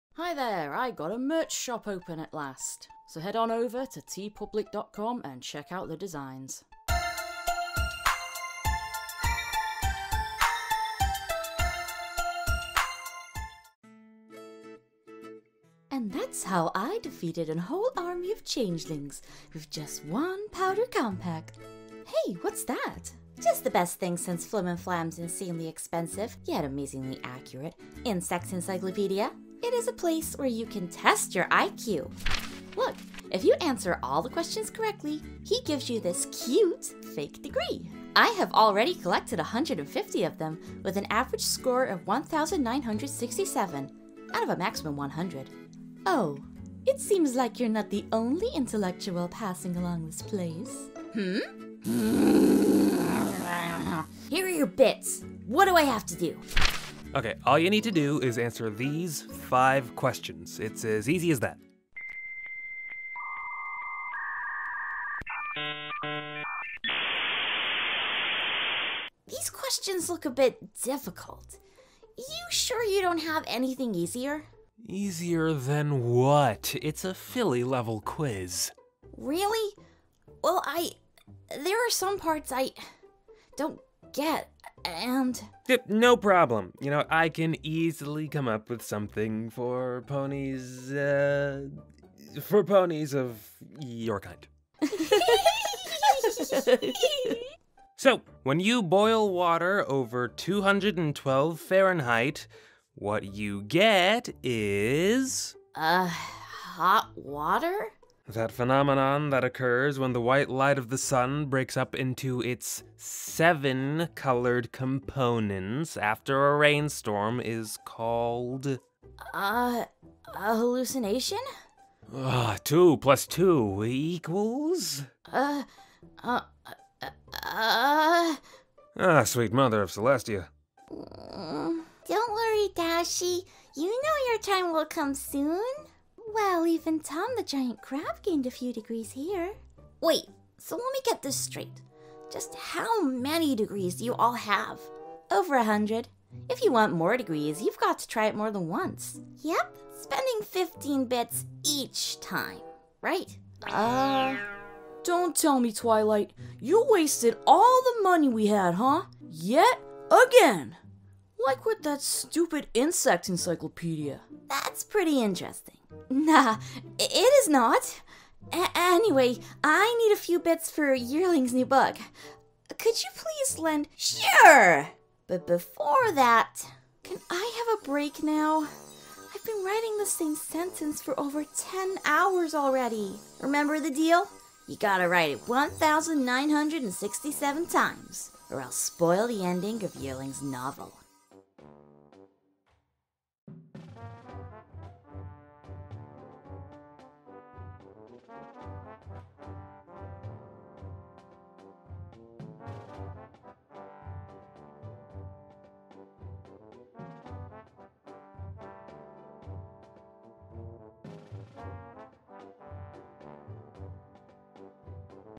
Test Your I Q - MLP Comic Dub (by labba94)
Summary: Rainbow Dash tests her I.Q with interesting results

Follow me on Tik-Tok - @Shelly.Kitten